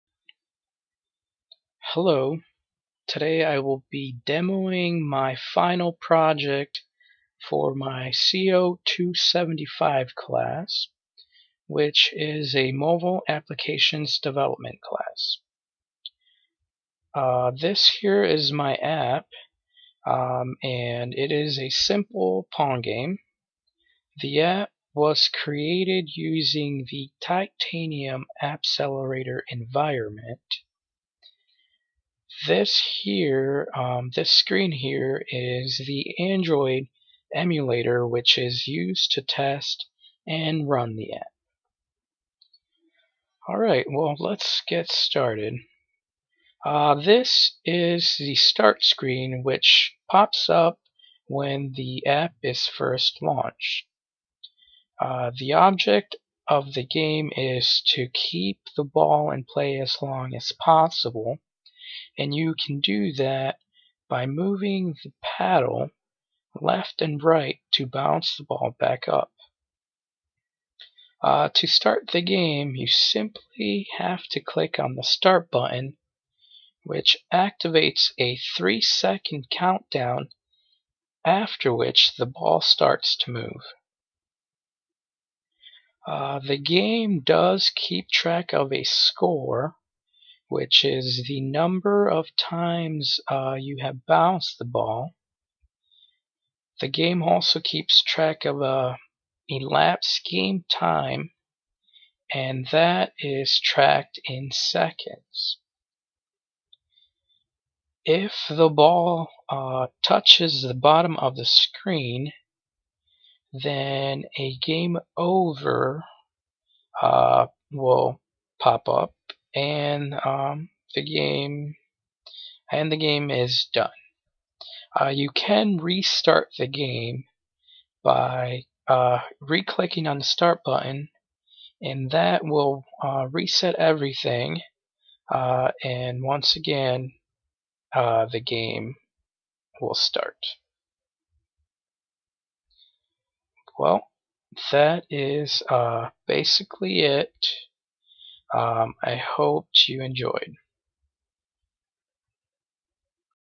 Pong Game (Titanium Appcelerator)
This is my final project for my CO 275 class at GRCC. It is a simple pong game that I created using the Titanium Mobile Development Platform.